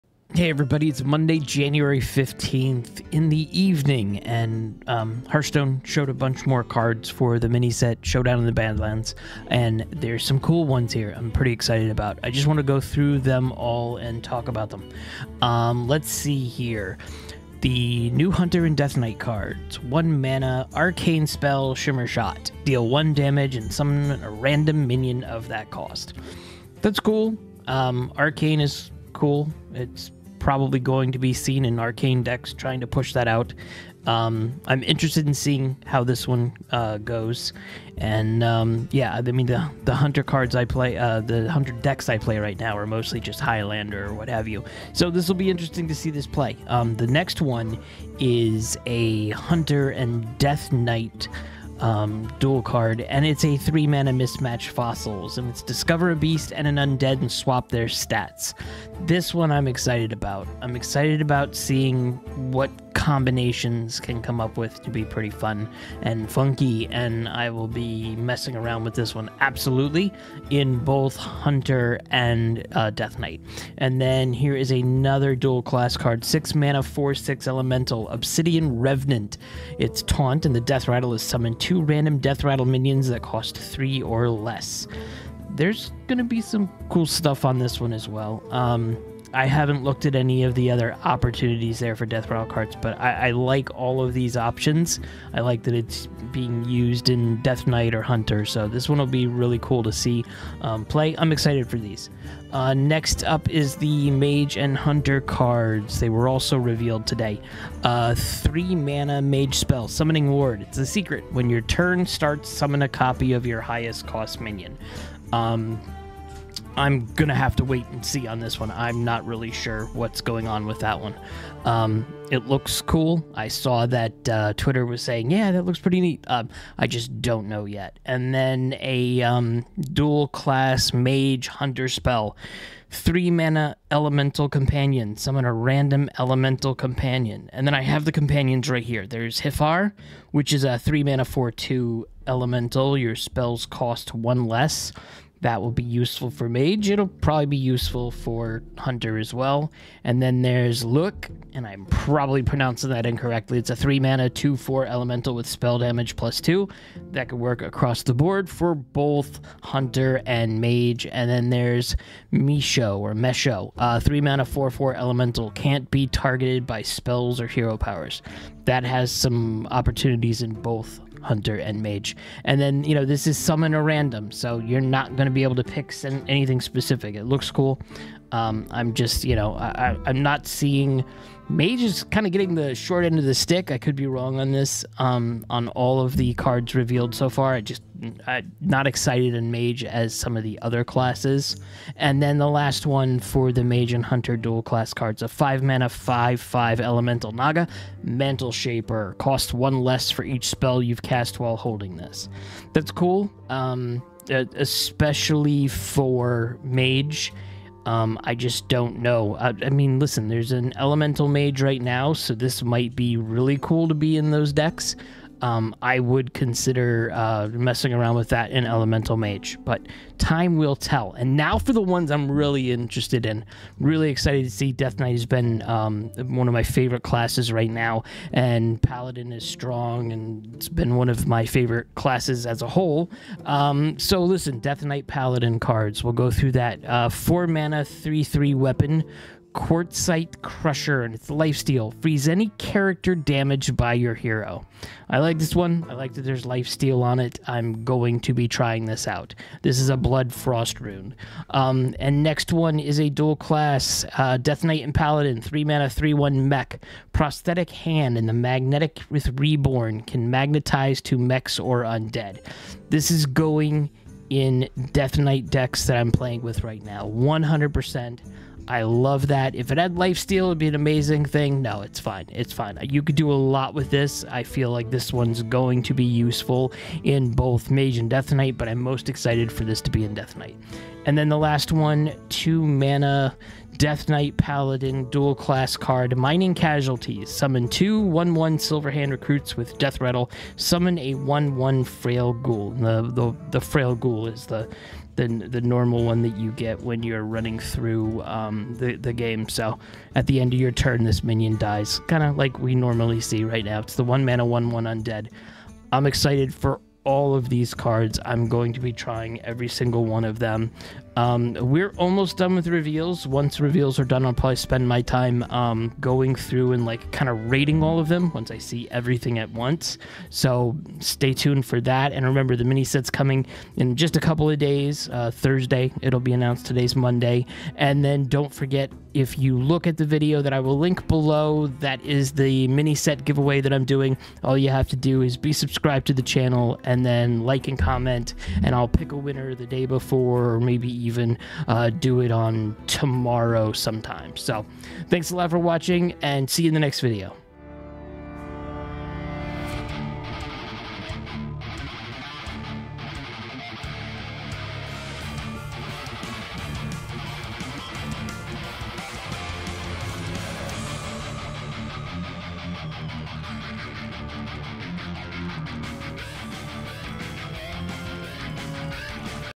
MORE! Hearthstone Mini-Set Cards Revealed! Death Knight Hunter and Mage!
More of the Hearthstone Delve into Deepholm Mini-Set cards have been revealed, and these are my favorite ones so far!!!